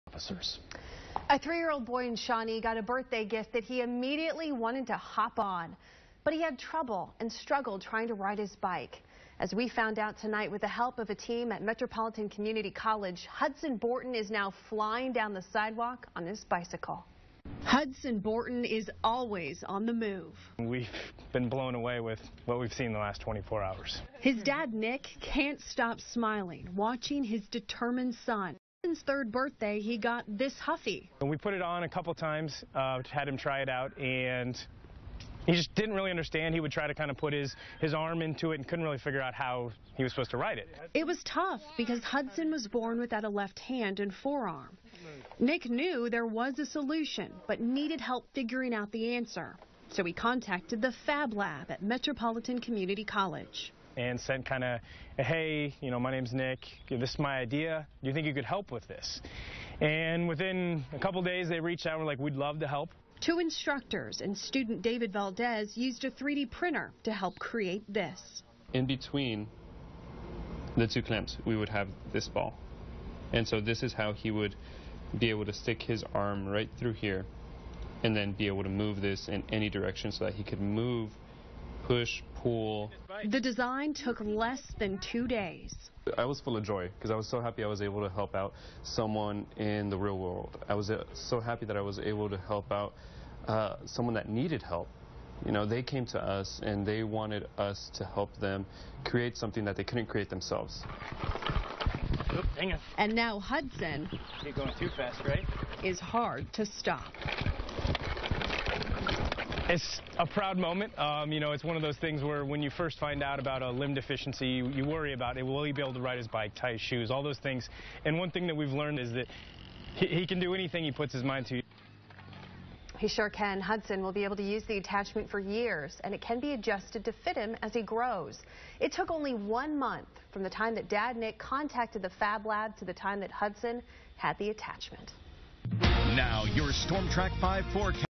KCTV 07 12 2016 MCC-Business & Technology helps boy ride bike
We change lives at MCC-Business & Technology. Take, for example, the story of Hudson Borton, a little boy born without his left hand. A crew in the Engineering Technology department - coordinator Mike Cline, instructor/lab tech Chris Page and student David Valdez - designed a prosthetic that allows Hudson to ride his first bike.